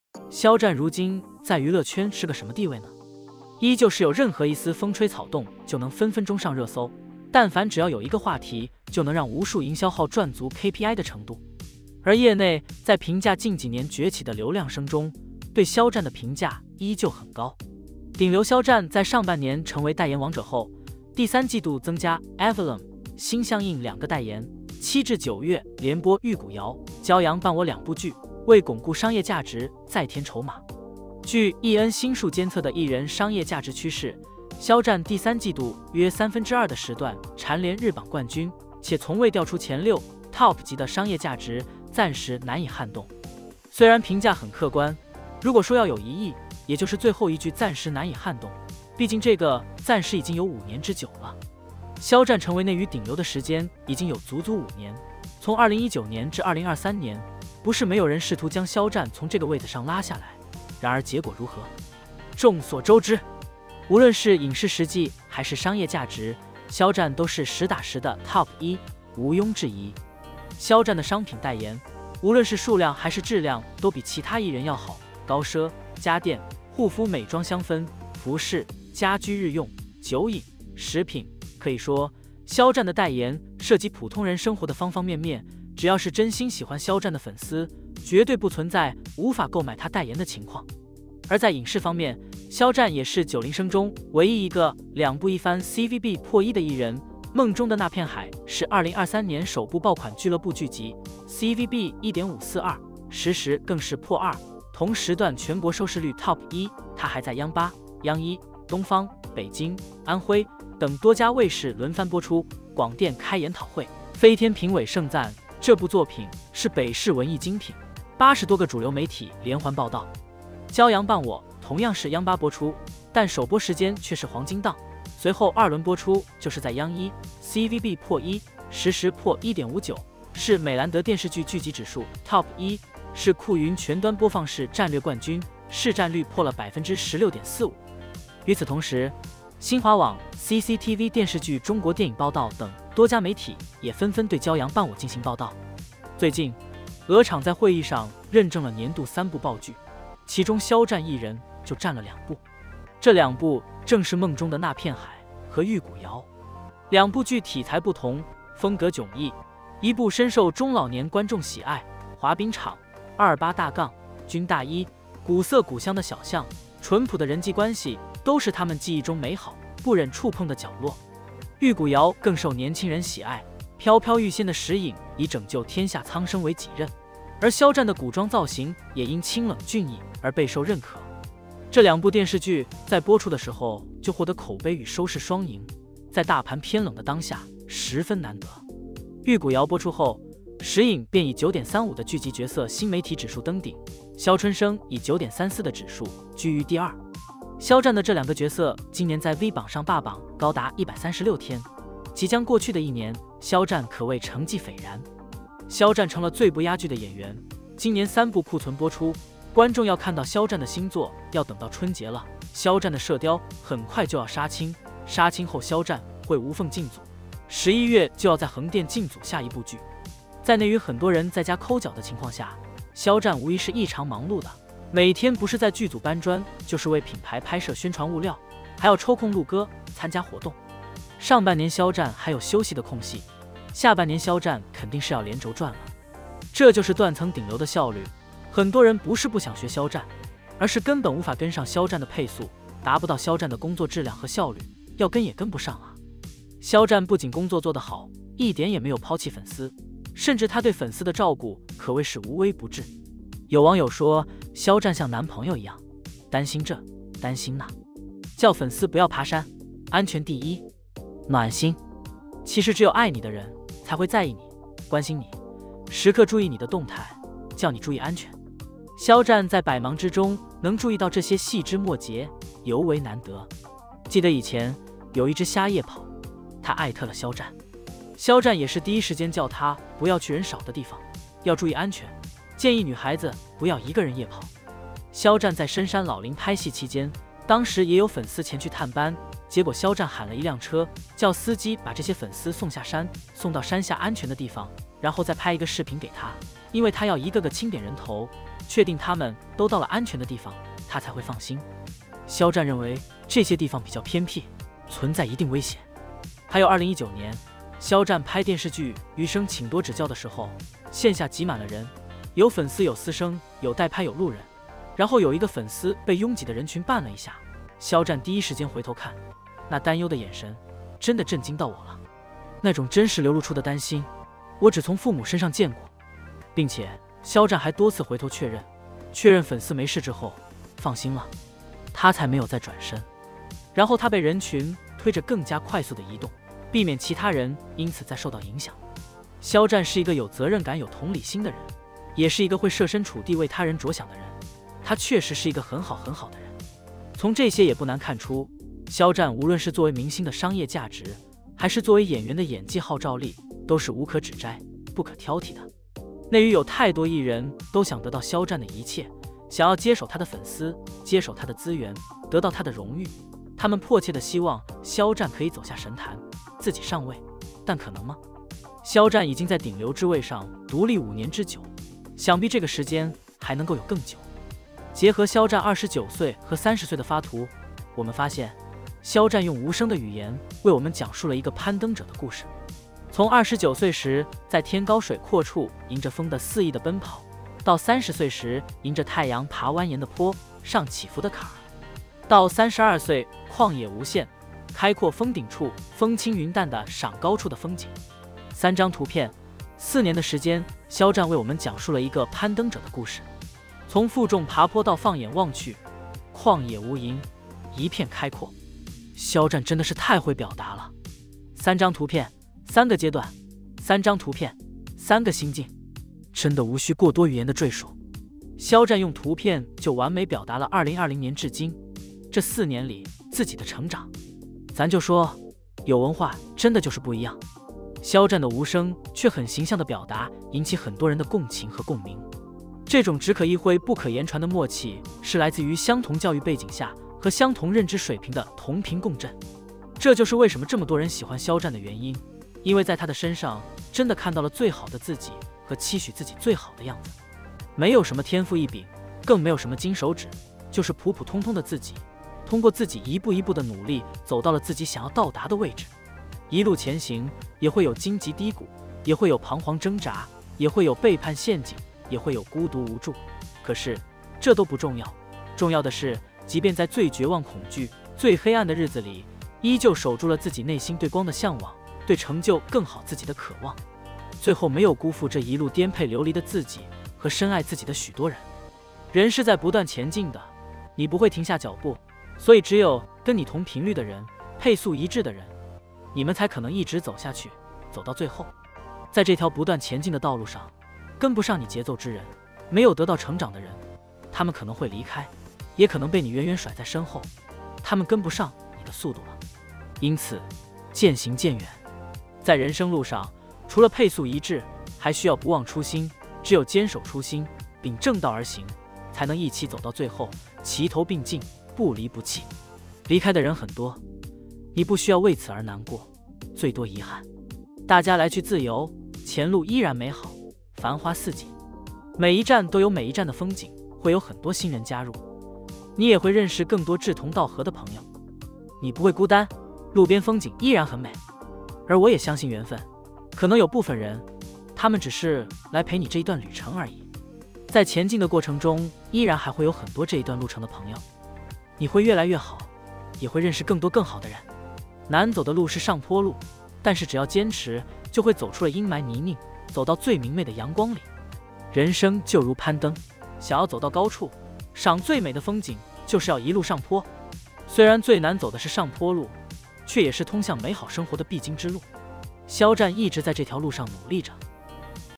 ENG SUB業內評價肖戰：暫時難以撼動！鵝廠會議認證爆劇，肖戰三佔二！肖戰關心粉絲發自內心
業內評價肖戰：暫時難以撼動！鵝廠會議認證爆劇，肖戰三佔二！肖戰關心粉絲發自內心